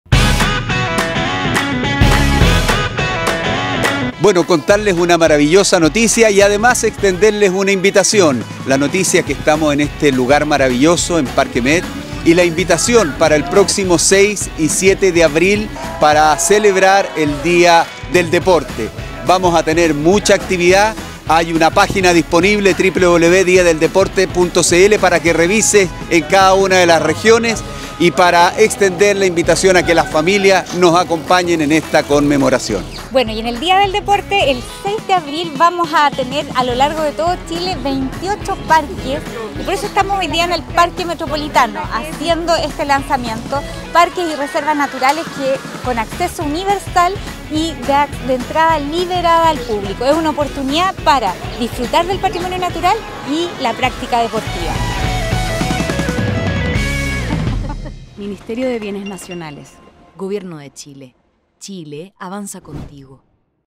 300 actividades y apertura gratuita de 28 Parques, Reservas y Monumentos Nacionales
La ministra Marcela Sandoval y el ministro Jaime Pizarronvitación para que el 6 y 7 de Abril puedan disfrutar de las más de 300 actividades y la apertura gratuita y universal de 28 Parques, Reservas y Monumentos Nacionales en el marco del DíaDelDeporte🏃‍♀️‍➡️